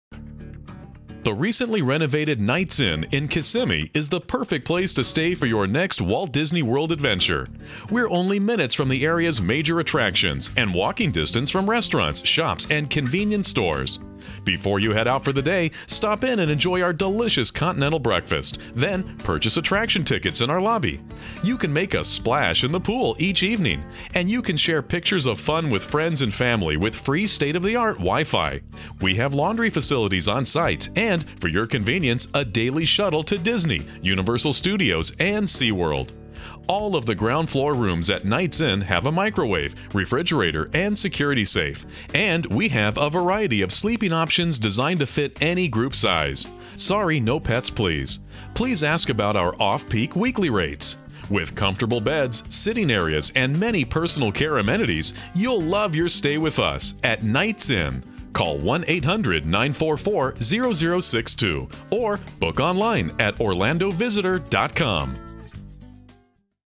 Knights Inn Kissimmee FL 34747
The recently renovated Knights Inn in Kissimmee is the perfect place to stay for your next Walt Disney World adventure. We’re only minutes from the area's major attractions, and walking distance from restaurants, shops, and convenience stores. Before you head out for the day, stop in and enjoy our delicious continental breakfast, then purchase attraction tickets in our lobby. You can make a splash in the pool each evening, and you can share pictures of the fun with friends and family with free state of the art Wi-Fi. We have laundry facilities on-site, and, for your convenience, a daily shuttle to Disney, Universal Studios and SeaWorld.  All of the ground floor rooms at Knights Inn have a microwave, refrigerator, and security safe, and we have a variety of sleeping options designed to fit any group size. Sorry, no pets, please. Please ask about our off-peak weekly rates! With comfortable beds, sitting areas, and many personal care amenities, you’ll love your stay with us at Knights Inn.